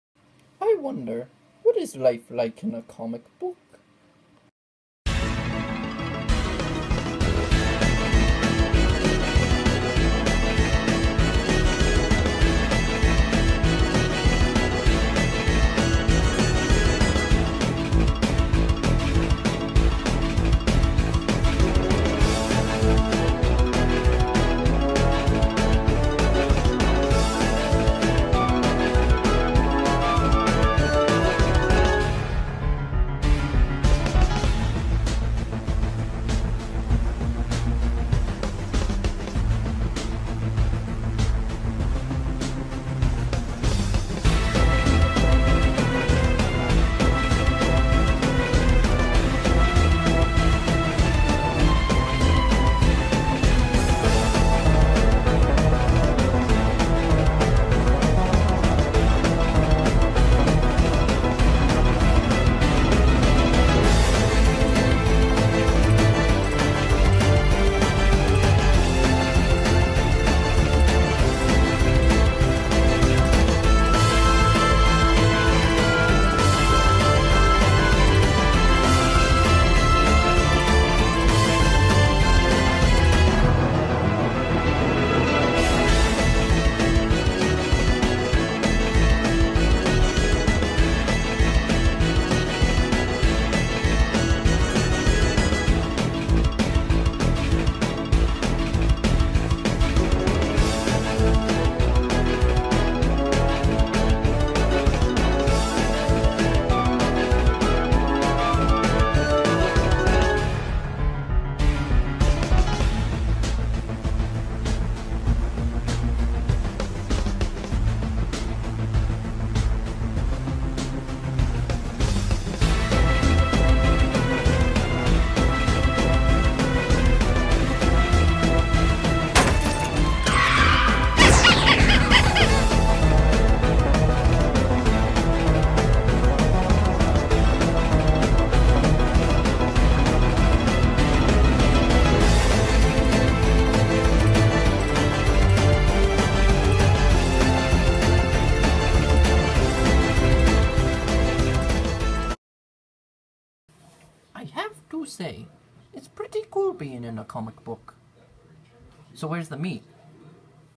Mario & Friends Shorts episode 13: Comic Book Special
Ever wanted to see Mario & Friends in a comic book? Here's a special episode made just for that reason!